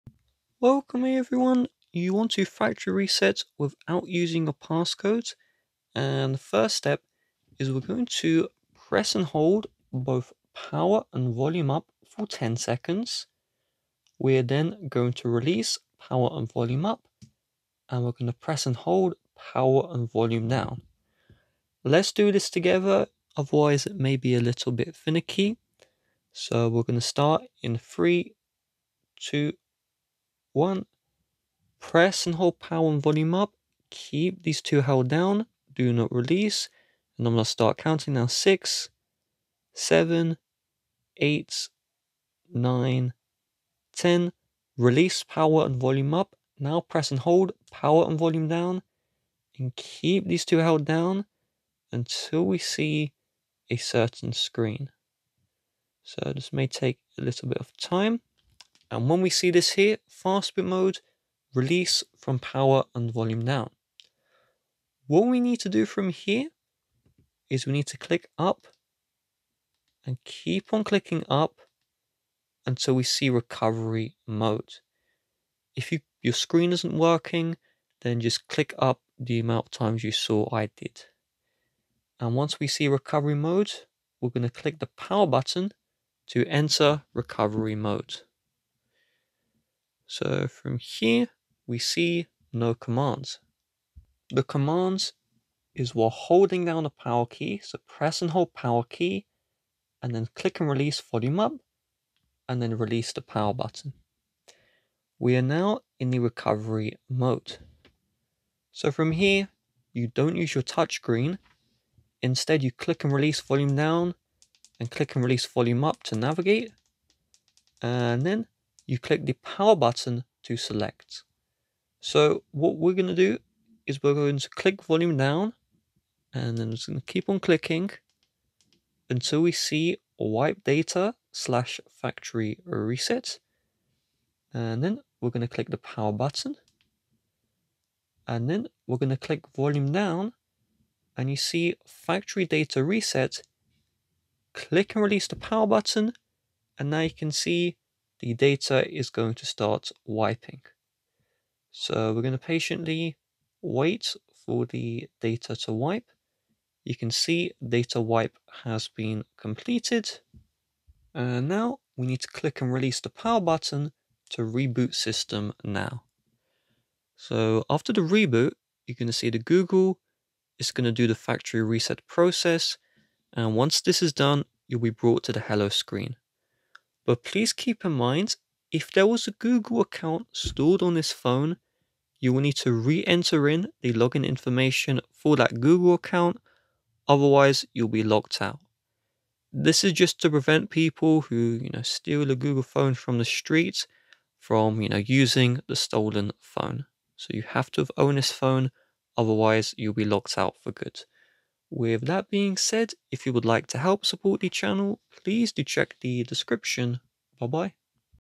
How To Factory Reset Google Pixel 8 WITHOUT Password!
Learn how to factory reset Google Pixel 8 without password.